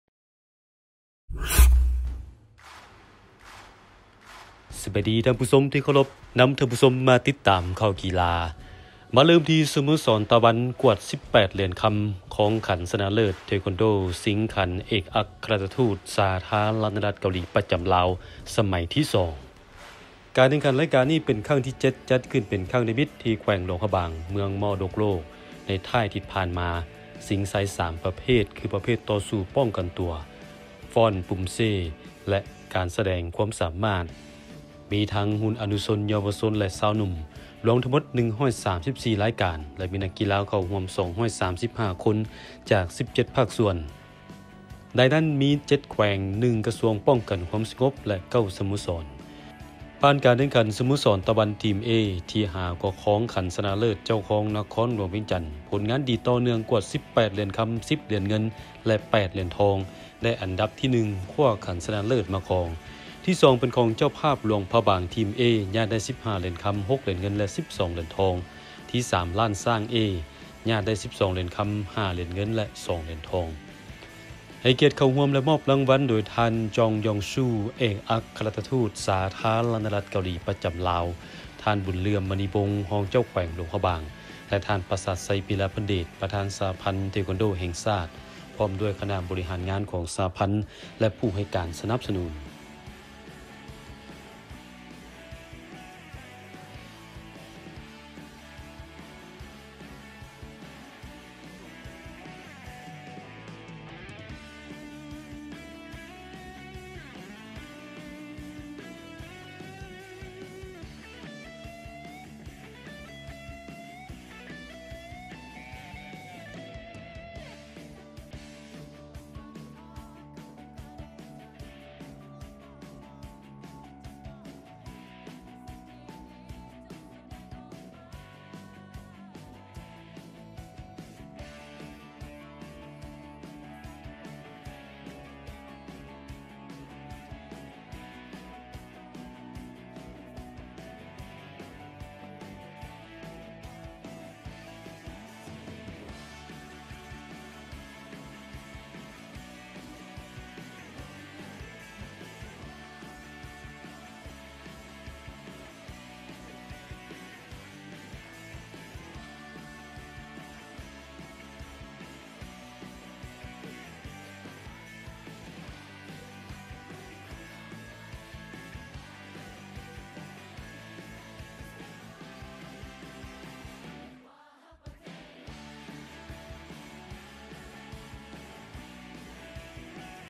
🎉🏆🥇🇱🇦 ສະໂມສອນ ຕະວັນ ຄອງຂັນຊະນະເລີດ ເທຄວັນໂດ້ ຊິງຂັນເອກອັກຄະລັດຖະທູດ ສ ເກົາຫລີ ປະຈຳລາວ ສະໄໝທີ 2
🎉🏆🥇🇱🇦 ສະໂມສອນ ຕະວັນ ກວາດ 18 ຫລຽນຄຳ ຄອງຂັນຊະນະເລີດ ເທຄວັນໂດ້ ຊິງຂັນເອກອັກຄະລັດຖະທູດ 🇰🇷 ສາທາະນະລັດເກົາຫລີ ປະຈຳລາວ ສະໄໝທີ 2